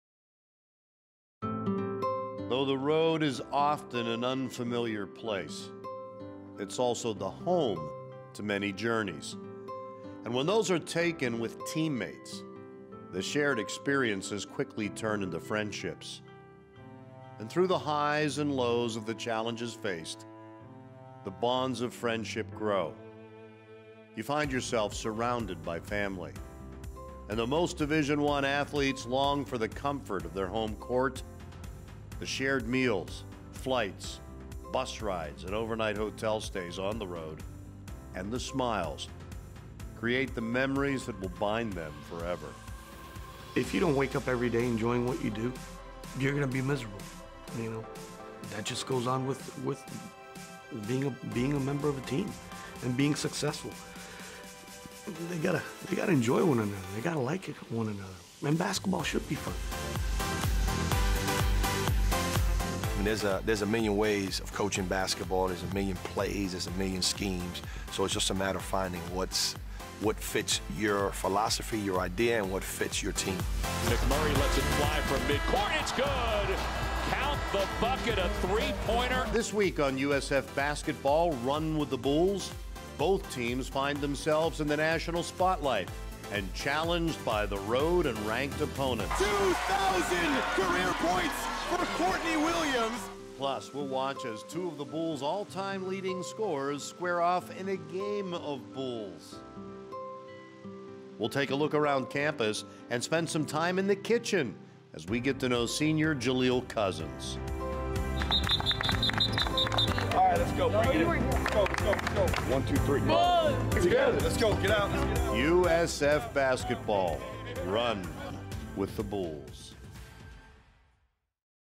USF BASKETBALL: Run with the Bulls - Episode 04 PREVIEW
Episode 4 preview:  Nationally ranked foes, bigs wins, 200 points for Williams, Cooking with Jaleel Cousins and 2 of the Bulls all time leading scorers square off in a game of B-U-L-L-S